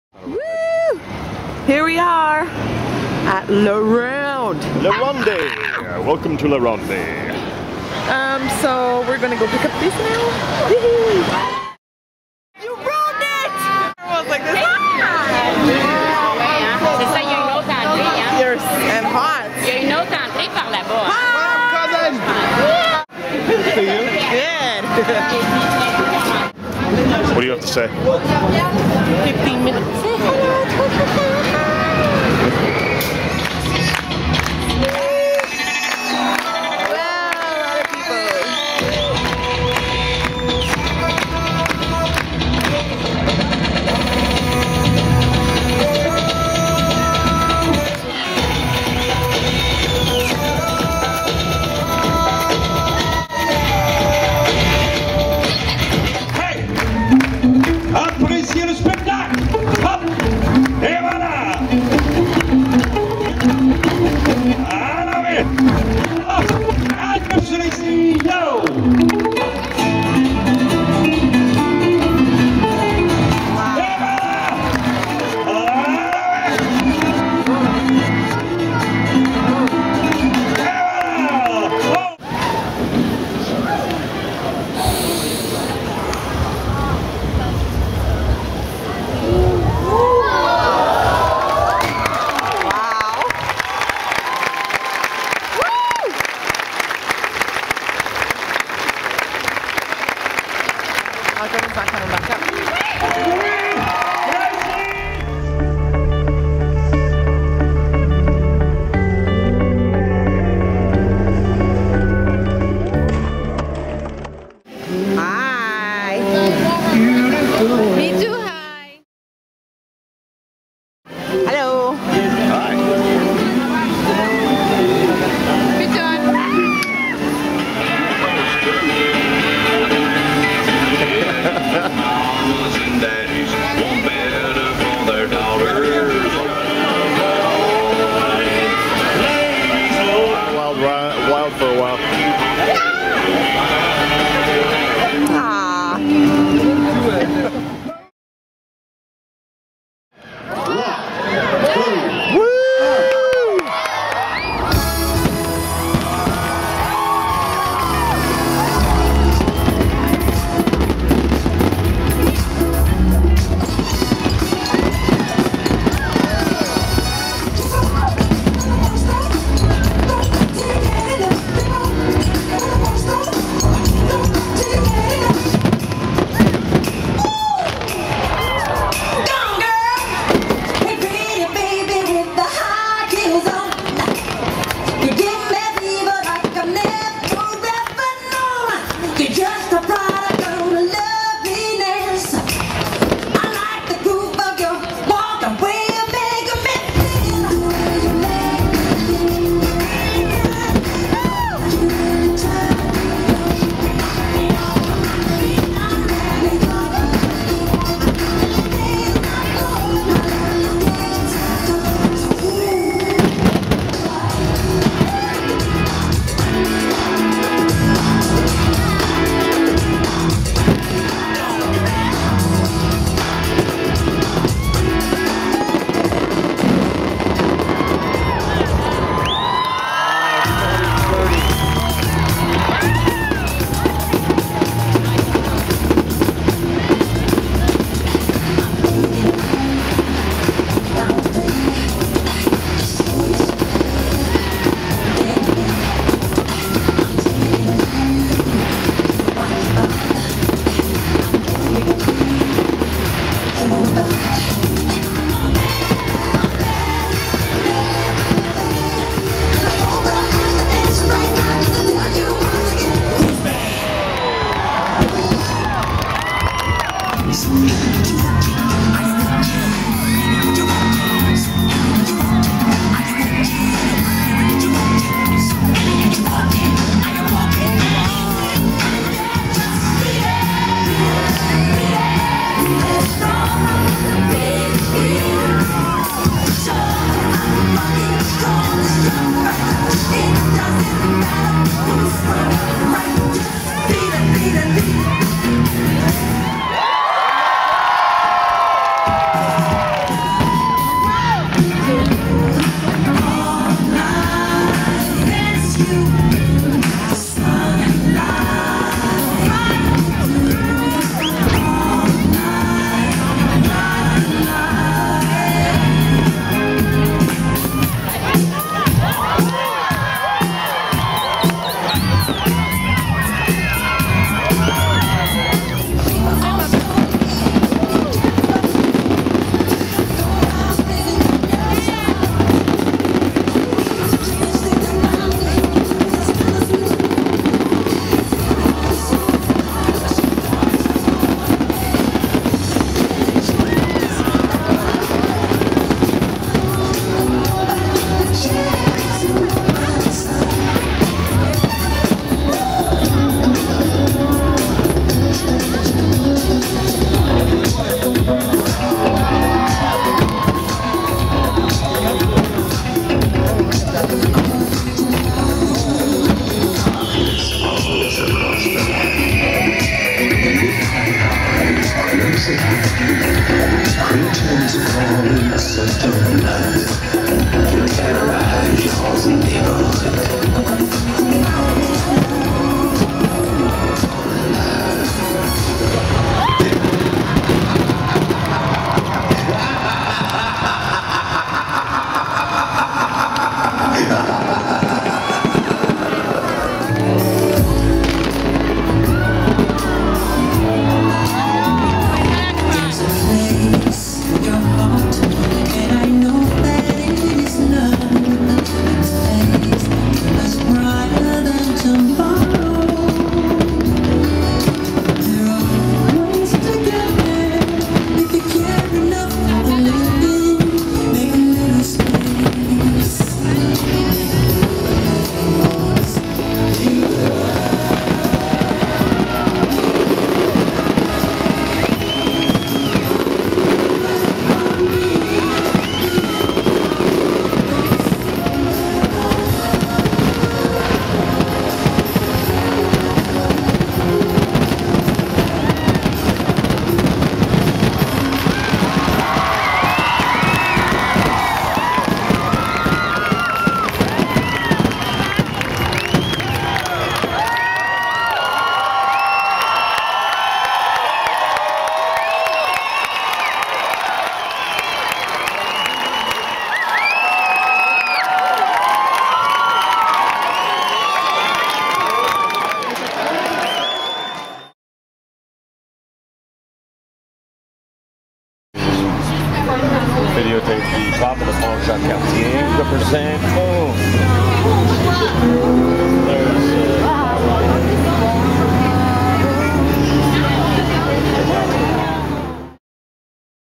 LaRonde Fireworks tribute to Michael Jackson...
These are just a few clips from our day at LaRonde! We had so much fun! ;p